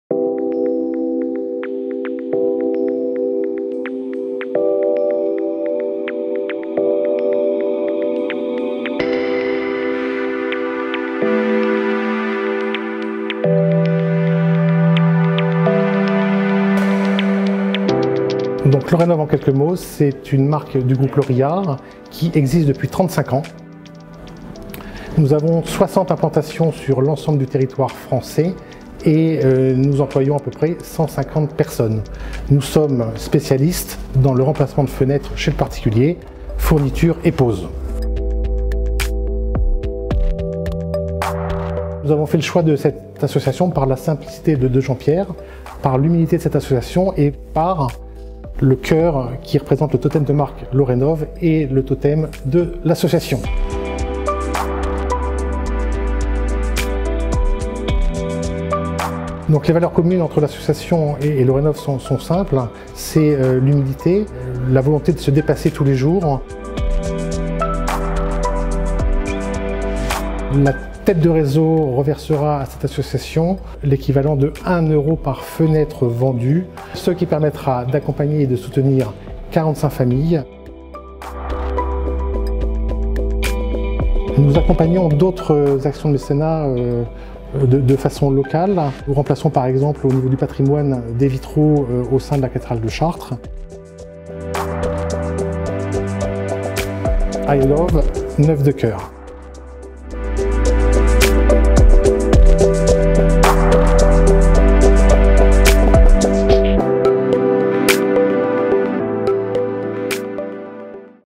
Interview de Pierre Lorillard : Neuf de Cœur, le choix du cœur !
Lorenove a décidé de soutenir l’association Neuf de Cœur, créée par Florence et Jean-Pierre Papin.

Pourquoi avoir choisi de soutenir Neuf de Cœur ?

- La simplicité de son fondateur, l’humilité de cette association nous ont tout de suite séduits.
- Coïncidence heureuse, l’association s’appelle Neuf de Cœur, alors que le cœur est la base même de notre totem de marque et le symbole graphique de notre communication (I ♥ Lorenove)

Quelles sont les valeurs communes entre Lorenove et Neuf de Cœur ?

Nos valeurs communes sont simples : La volonté de se dépasser tous les jours, le partage, l’entraide et les valeurs humaines.

Que fait Lorenove pour soutenir Neuf de Cœur ?

Le réseau national, spécialiste du remplacement de menuiseries, reversera à l’association, 1 € par fenêtre Lorenove achetée, ce qui permettra d’accompagner et soutenir environ 45 familles par an.

Vous aussi, engagez-vous aux côtés de Lorenove pour Neuf de Cœur !

Vous pouvez également nous aider à diffuser auprès de votre entourage, la cagnotte dédiée à cette opération

Merci !